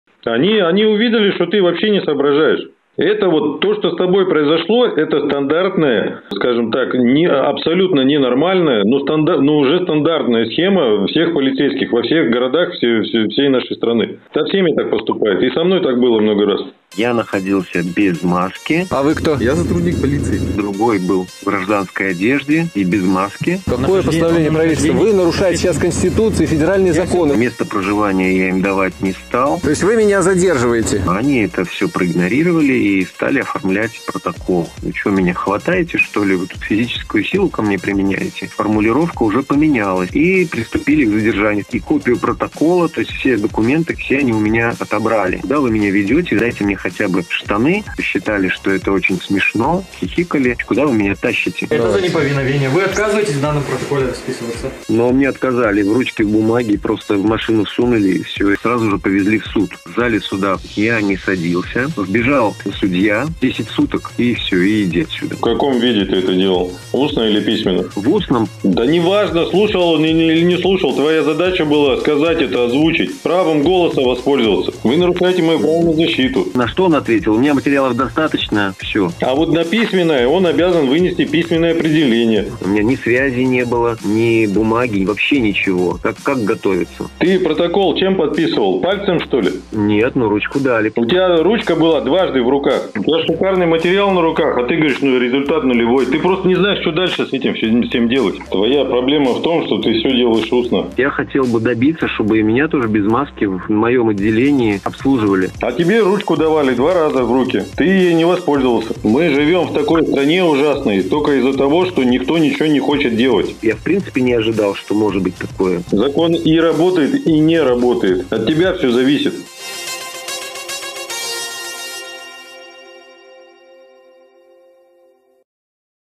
Трейлер ДВУЛИКИЙ ЗАКОН или НУЛЕВОЙ РЕЗУЛЬТАТ 2022.01.05 Сургут
разговор про задержание и протокола по статьям 20.6.1 и 19.3 КоАП

на ты или на вы?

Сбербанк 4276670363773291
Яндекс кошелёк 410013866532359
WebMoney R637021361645